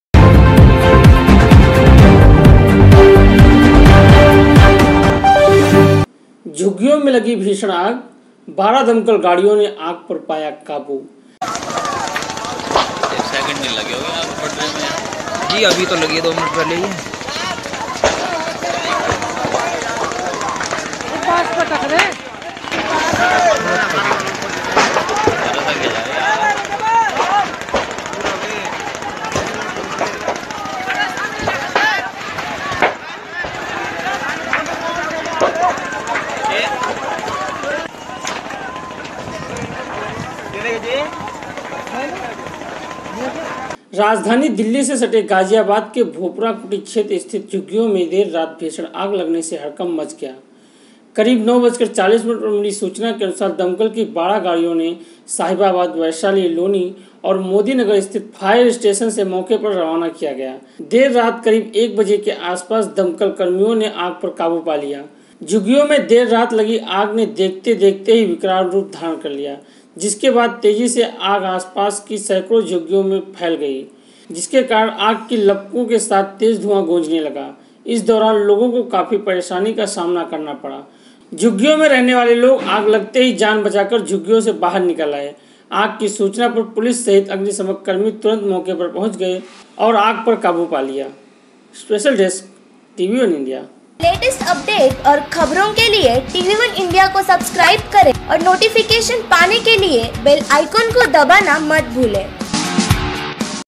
झुग्गियों में लगी भीषण आग | 12 दमकल गाड़ियों ने आग पर पाया काबू
झुग्गियों में लगी भीषण आग | 12 दमकल गाड़ियों ने आग पर पाया काबू | राजधानी दिल्ली से सटे गाजियाबाद केभोपुरा कुटी क्षेत्र स्थित झुग्गियों में मंगलवार देर रात भीषण आग लगने से हड़कंप मच गया... रात करीब 9:40 बजे सूचना मिलने पर दमकल की 12 गाड़ियों को साहिबाबाद, वैशाली, लोनी और मोदीननगर स्थित फायर स्टेशन से मौके पर रवाना किया गया...